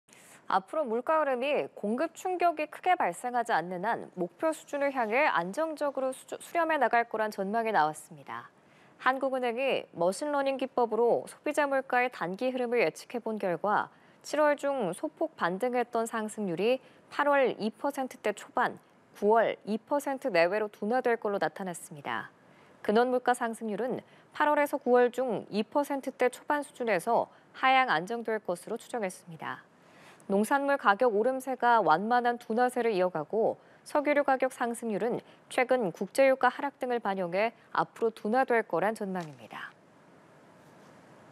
한은 "9월 소비자물가 상승률 2% 내외까지 둔화될 듯" / 연합뉴스TV (YonhapnewsTV)
한은 "9월 소비자물가 상승률 2% 내외까지 둔화될 듯"

앞으로 물가 흐름이 공급충격이 크게 발생하지 않는 한 목표 수준을 향해 안정적으로 수렴해나갈 거란 전망이 나왔습니다.

한국은행이 머신러닝 기법으로 소비자물가의 단기 흐름을 예측해본 결과, 7월 중 소폭 반등했던 상승률이 8월 2%대 초반, 9월 2.0% 내외로 둔화될 것으로 나타났습니다.

근원물가 상승률은 8~9월 중 2%대 초반 수준에서 하향 안정될 것으로 추정했습니다.

농산물 가격 오름세가 완만한 둔화세를 이어가고, 석유류 가격 상승률은 최근 국제유가 하락 등을 반영해 앞으로 둔화될 거란 전망입니다.